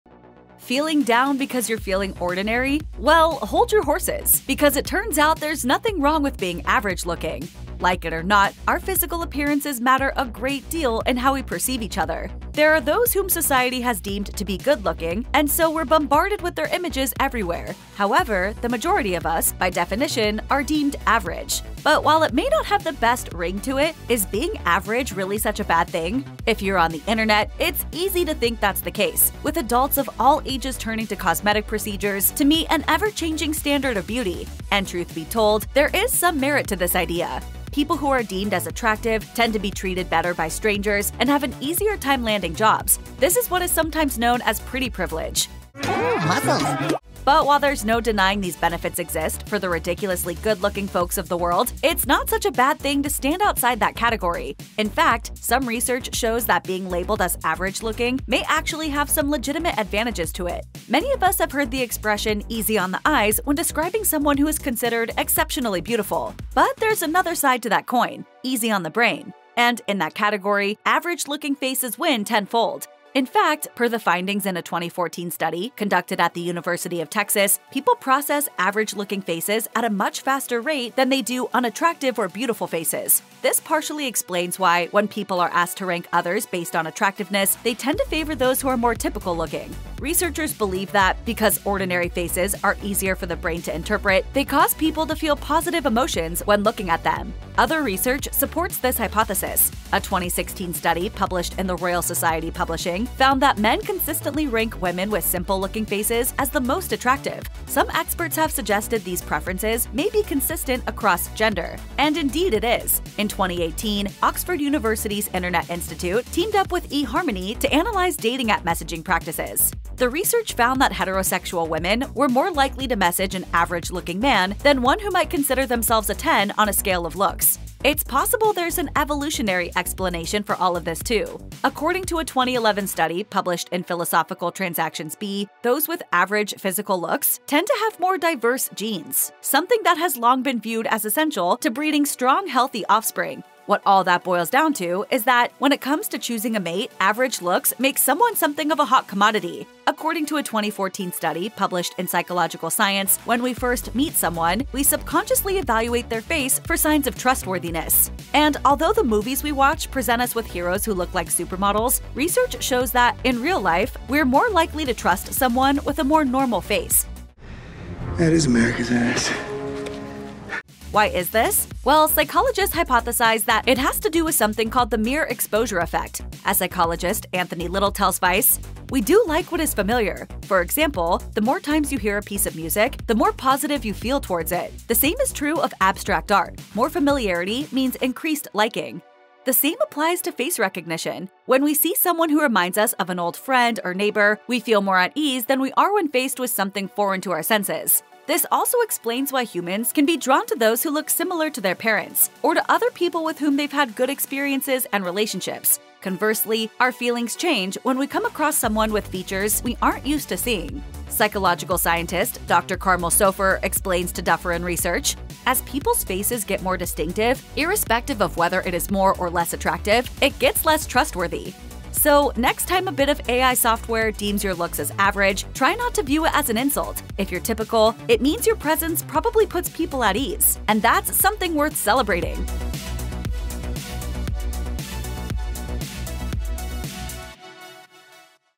Why 'Average' Looks Are Actually An Advantage
Feeling down because you're feeling ordinary? Well, hold your horses — because it turns out there's nothing wrong with being average-looking.